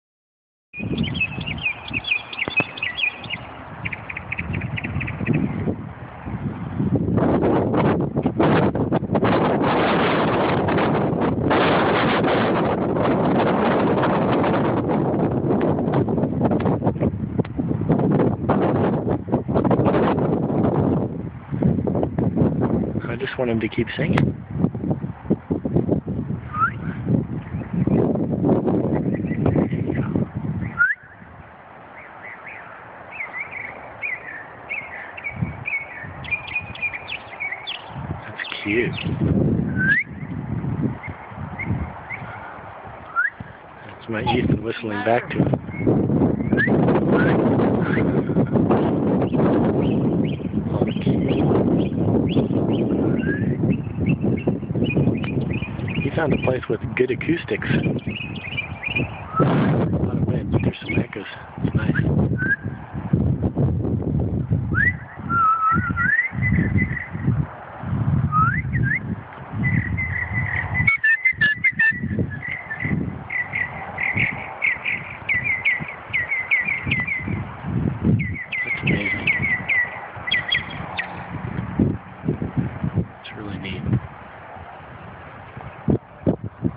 Good Accoustics - some bird
Magpie?  I don't have a clue.  Something with a long tail singing it's heart out cutely.